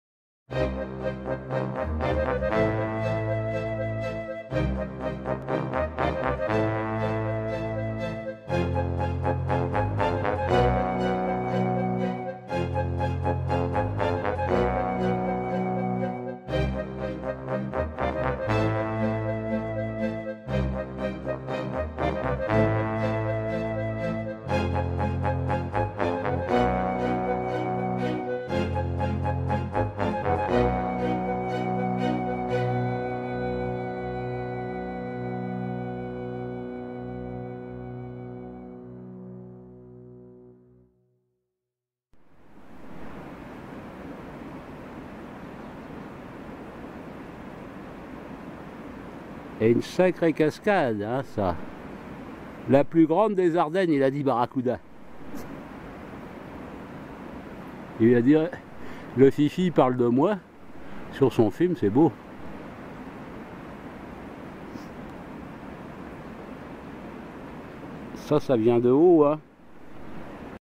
La Cascade
Située sur la rive opposée à la Voie Verte Trans-Ardennes et proche du pont de Laifour, cette cascade n'est visible qu'à la fonte des neiges ou lors de pluies torrentielles. En dehors de la période hivernale, elle est masquée par la végétation et est inaccessible.